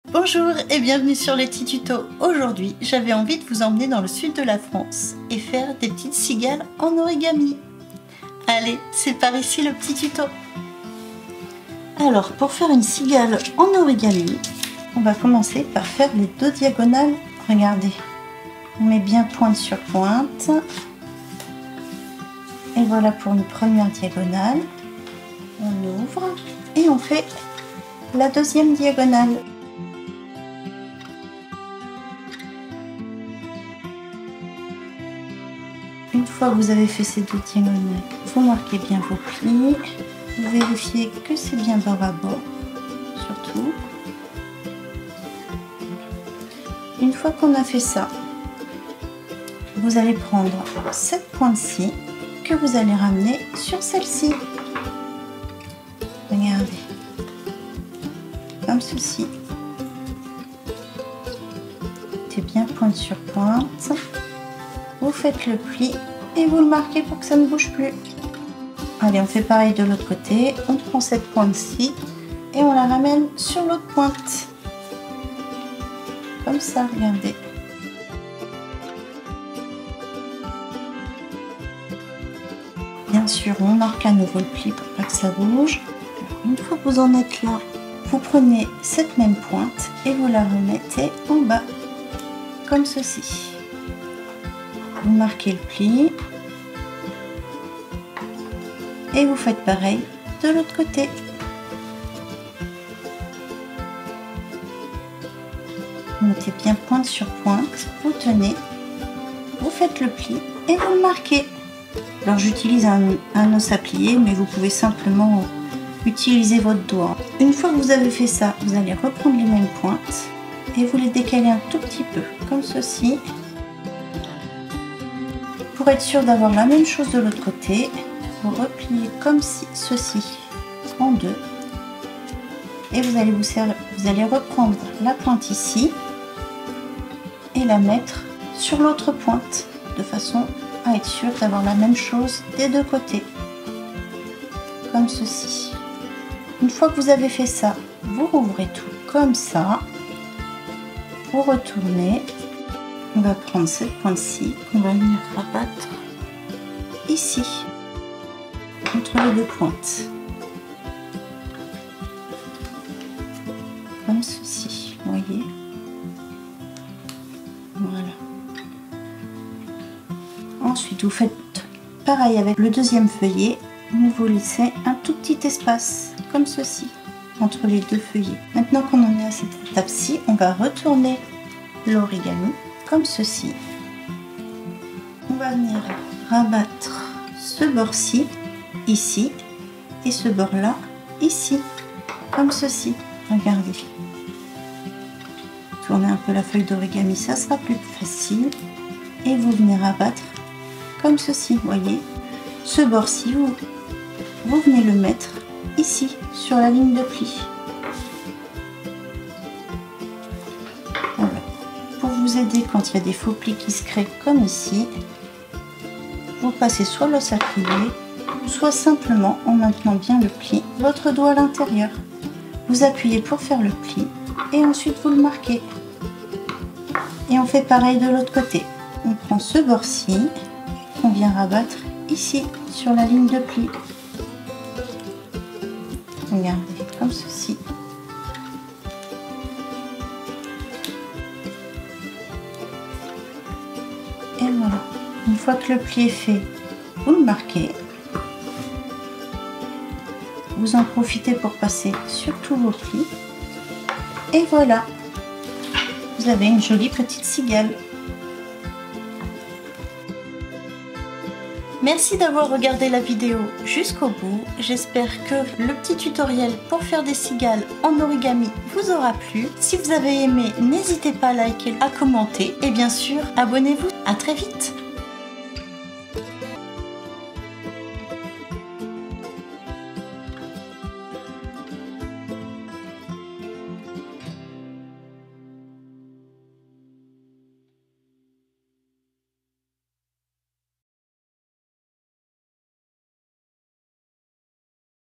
Origami cigale : plier une cigale en papier (Tuto Facile)
J’espère que vous passez de bonnes vacances, aujourd'hui je vous emmène dans le sud de la France avec cette cigale en origami toute mimi, le saviez-vous on peut entendre son chant à partir de fin juin à mi-septembre.
kss Ksss Ksss !

Pour réaliser cette jolie cigale en papier, il vous faudra une feuille d'origami ou une feuille mise au format carré de préférence bicolore, mais après libre à vous de choisir bicolore, à motif ou uni.

Fournitures pour réaliser cet origami

Papier origami Clairefontaine 15x15cm Creativ’ Paper - Collection Nénuphars

Os à plier pour l'origami - Plioir en plastique